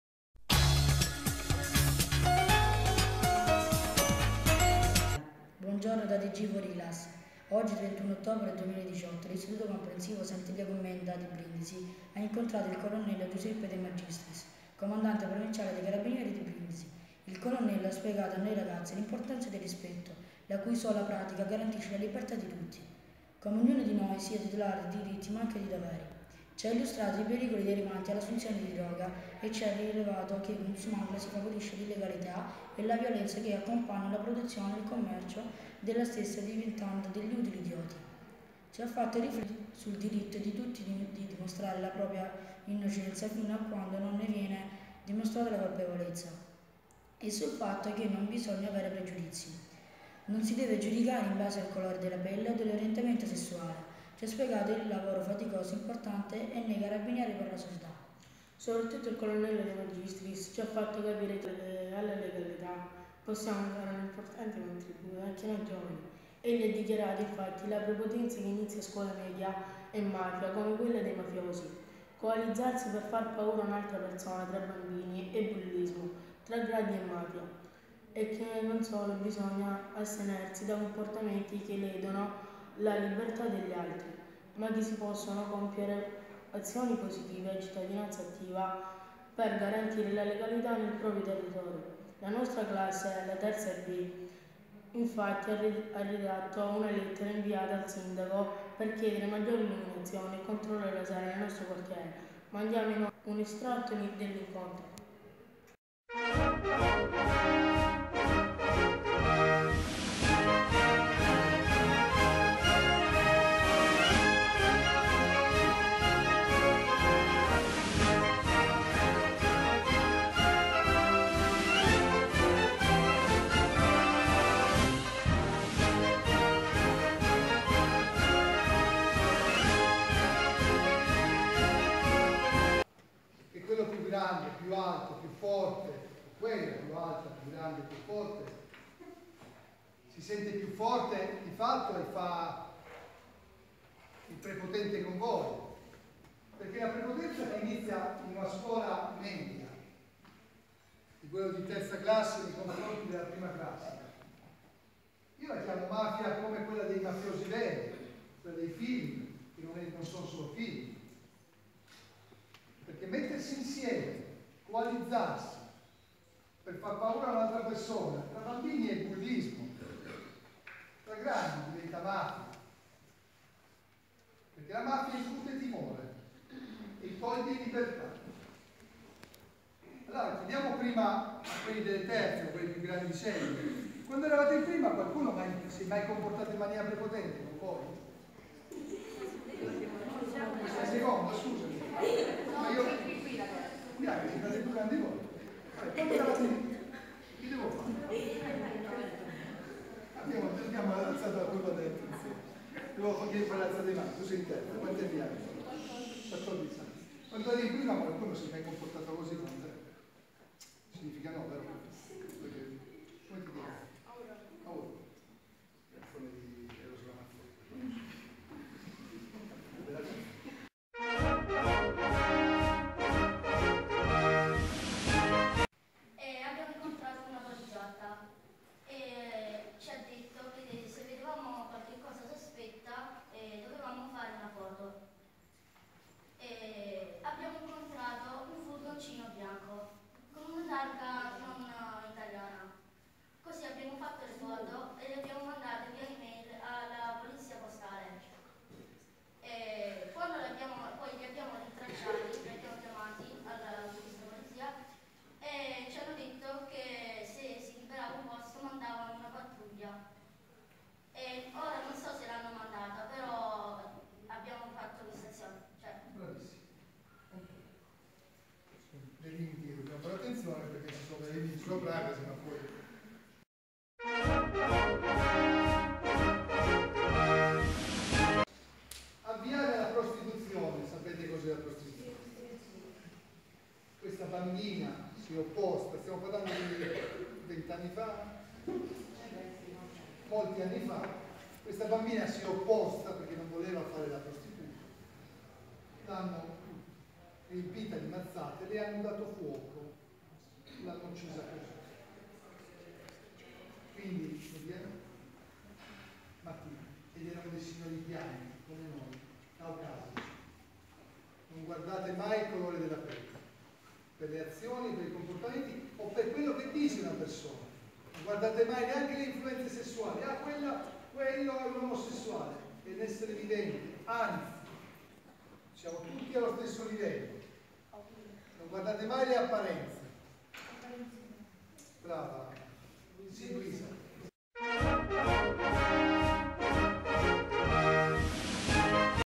Incontro con il Colonnello Giuseppe De Magistris, comandante provinciale dei Carabinieri di Brindisi
IC Sant'Elia-Commenda di Brindisi incontra il Colonnello Giuseppe De Magistris, comandante provinciale dei Carabinieri di Brindisi - 31-10-18